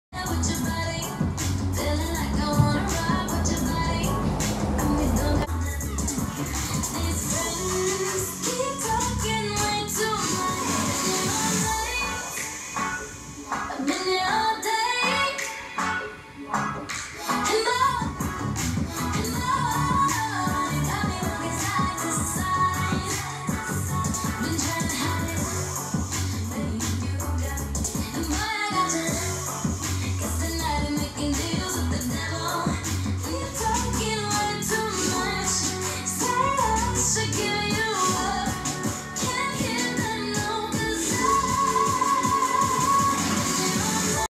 20 min AMRAP
Grab your dumbbells for this workout targeting your upper body and core!
20 minutes As Many Rounds As Possible, 10 reps of each exercise:
Renegade Rows
Pushups
Leg Levers
Shoulder Press
Burpees